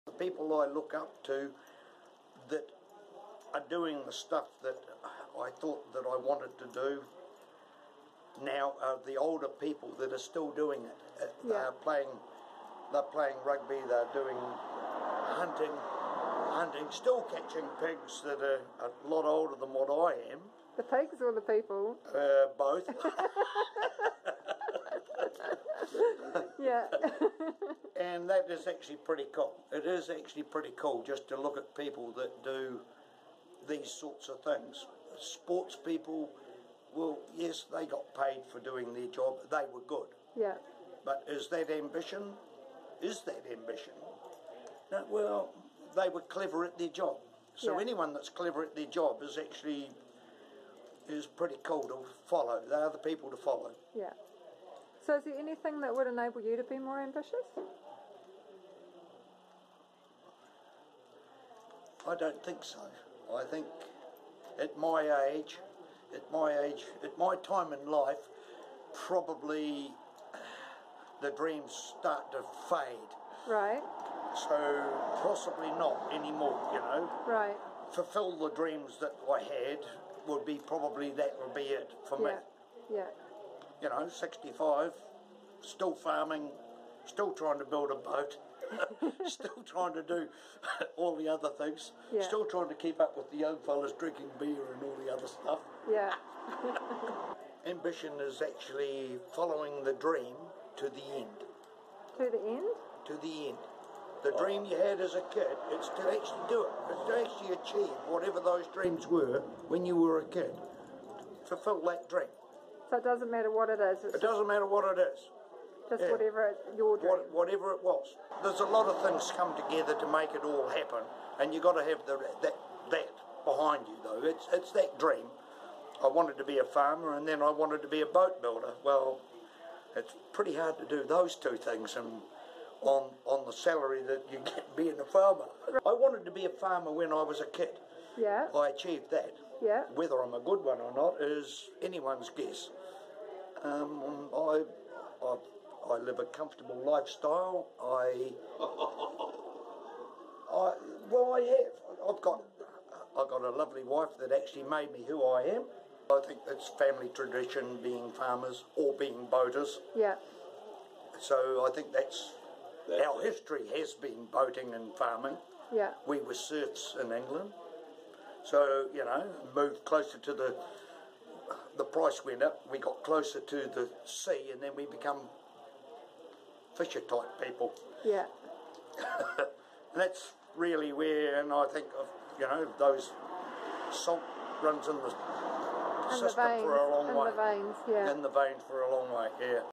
Harry Trewavas
Farmer Harry Trewavas says ambition is following the dream to the end.